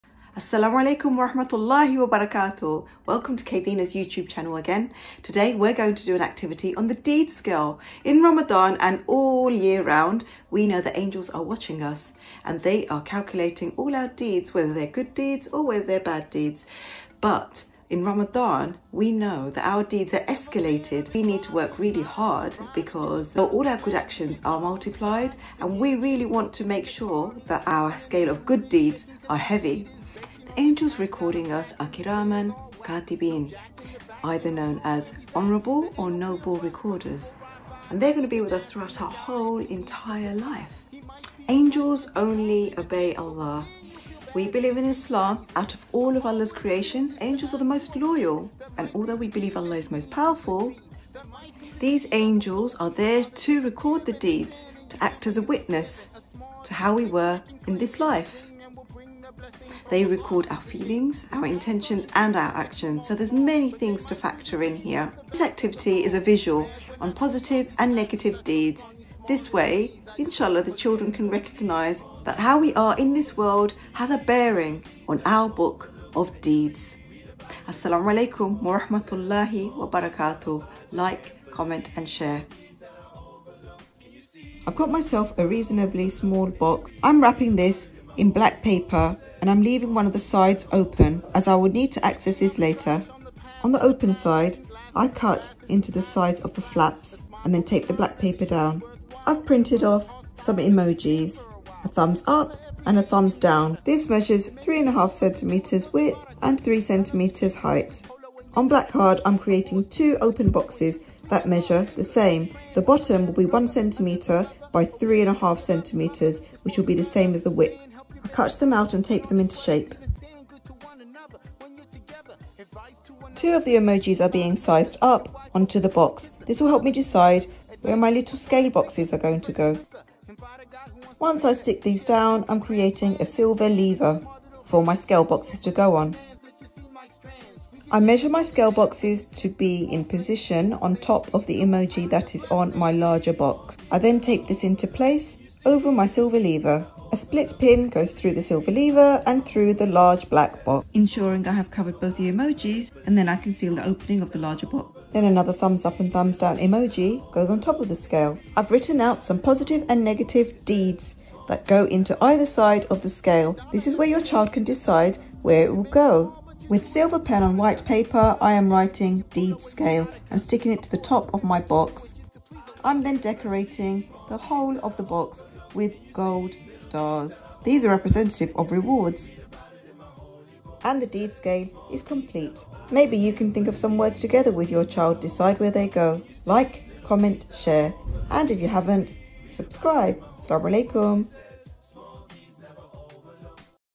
How to Make a Deeds Scale
Teach your children about our scale of deeds that angels record throughout life. In Ramadan our good deeds are maximised.

After making this craft, they can establish what are positive and what are negative deeds and work towards getting heavier on the scale of good deeds.